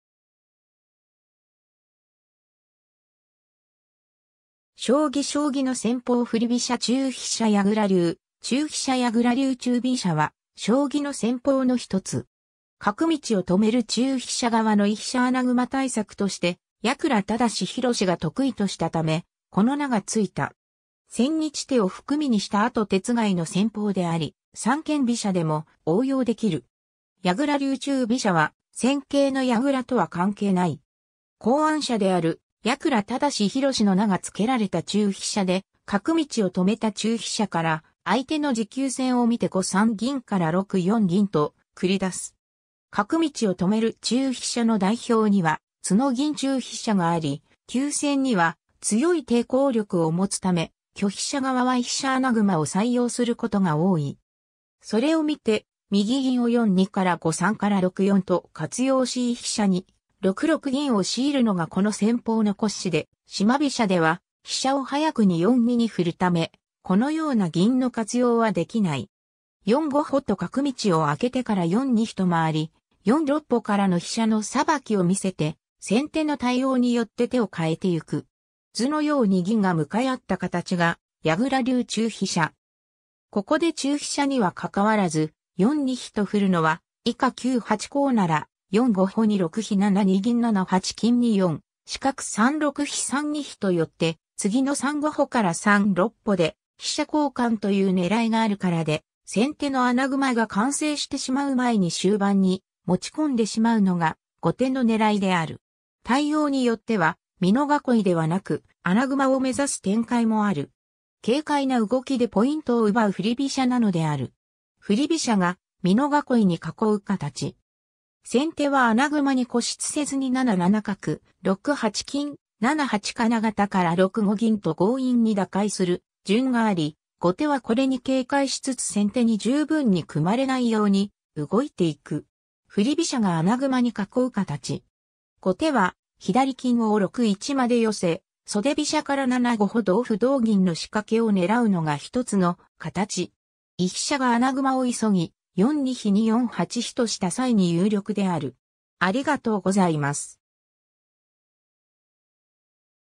矢倉流中飛車
将棋 」 将棋の戦法 」 振り飛車 」 中飛車 」 矢倉流中飛車 矢倉流中飛車は将棋の戦法の1つ。
角道を止める中飛車側の居飛車穴熊対策として矢倉規広が得意としたため、この名がついた。
千日手を含みにした後手番の戦法であり、三間飛車でも応用できる。
 矢倉流中飛車は戦型の矢倉とは関係ない。
考案者である矢倉規広の名が付けられた中飛車で、角道を止めた中飛車から、相手の持久戦を見て△5三銀から6四銀と繰り出す。
 角道を止める中飛車の代表にはツノ銀中飛車があり、急戦には強い抵抗力を持つため（居飛車側にも加藤流袖飛車や4六金戦法等の有力な急戦はある）、居飛車側は居飛車穴熊を採用することが多い。
それをみて右銀を4二～5三〜6四と活用し居飛車に▲6六銀を強いるのがこの戦法の骨子で（△6五銀～△7六銀を防ぐ）、四間飛車では飛車を早くに4二に振るため、このような銀の活用は出来ない。
△4五歩と角道を開けてから△4二飛と回り、△4六歩からの飛車の捌きを見せて先手の対応によって手を変えていく。
 図のように銀が向かい合った形が矢倉流中飛車。
ここで中飛車には拘らず、△4二飛と振るのは、以下▲9八香なら、△4五歩▲2六飛△7二銀▲7八金に△4四角▲3六飛△3二飛と寄って、次の△3五歩～3六歩で飛車交換という狙いがあるからで、先手の穴熊が完成してしまう前に終盤に持ち込んでしまうのが後手の狙いである。
 対応によっては美濃囲いではなく穴熊を目指す展開もある。
軽快な動きでポイントを奪う振り飛車なのである。
 振り飛車が美濃囲いに囲う形。
先手は穴熊に固執せずに▲7七角・▲6八金・▲7八金型から▲6五銀と強引に打開する順があり、後手はこれに警戒しつつ先手に十分に組まれないように動いていく。
 振り飛車が穴熊に囲う形。
後手は左金を6一まで寄せ、袖飛車から△7五歩▲同歩△同銀の仕掛けを狙うのが1つの形。
居飛車が穴熊を急ぎ、△4二飛に▲4八飛とした際に有力である。